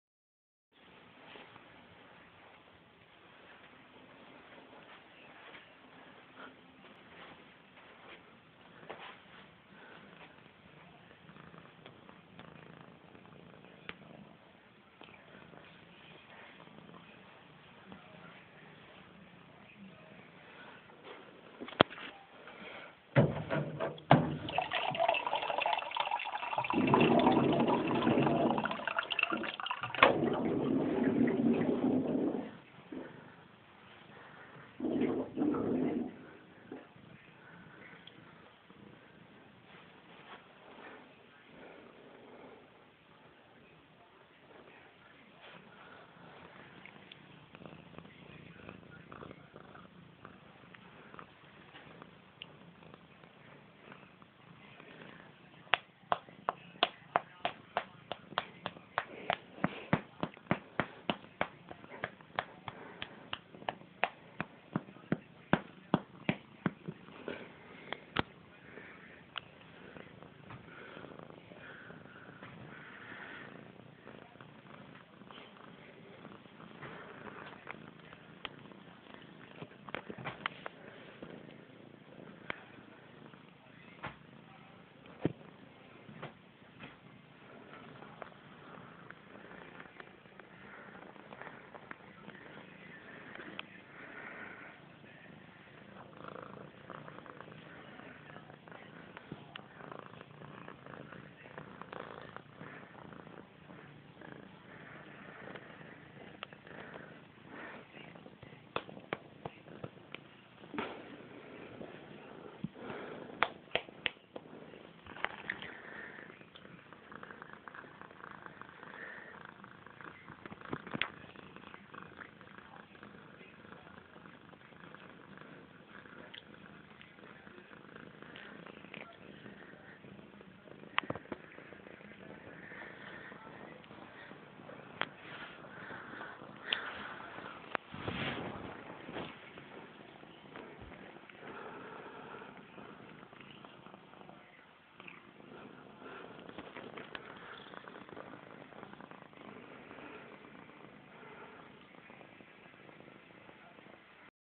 Cat (Missy Grey) as I get drink of water.
As I was getting a drink of water I saw Missy Grey napping on my bed so I decided to give her some ''finger flicks'' on her belly because she likes it so much.